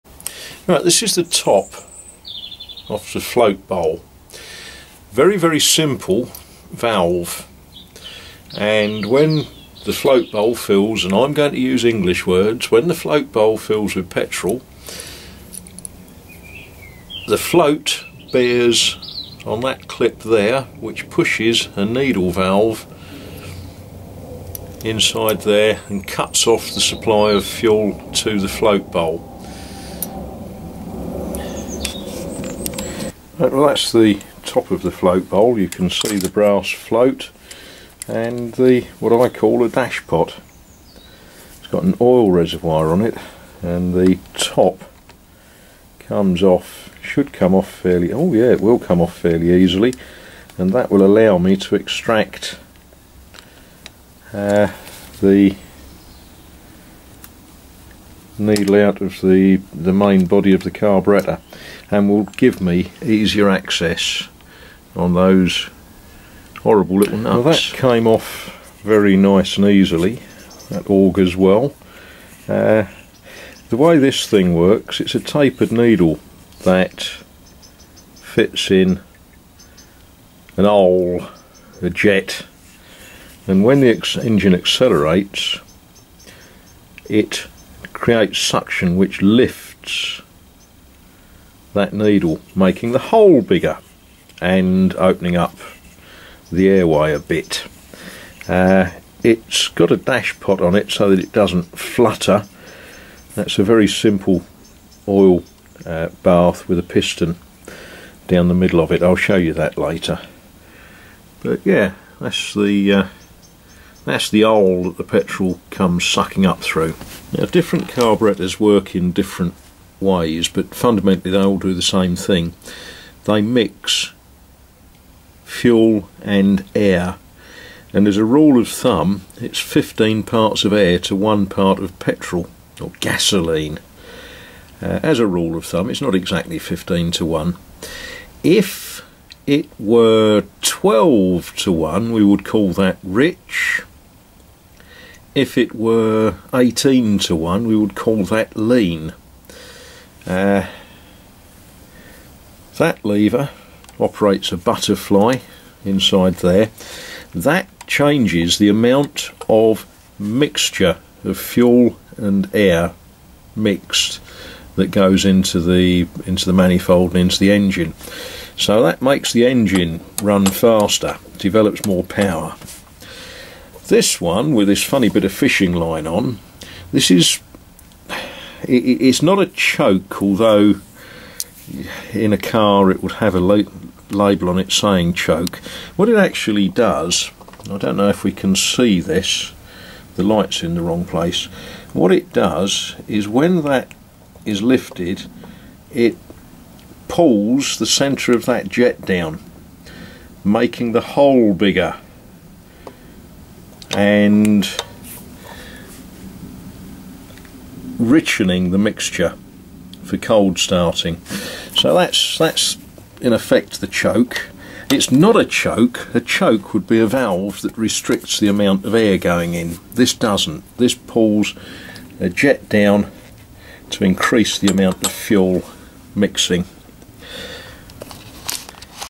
Exploring an SU carburettor
Carburettors seem to be a 'thing of the past', so that cars can conform with Californian laws on emissions, but for many decades, they allowed the average mechanic to adjust engines to run very efficiently.
All they do is mix fuel with air, metering the mixture to get the best 'burn', and controlling the amount of the mixture that goes into the cylinders.
The SU types of carburettors are to be found on Minis, Morris Minors and many classic British cars. No surprise then to find one on an Austin Nautilus, although I think this one is much younger than the engine.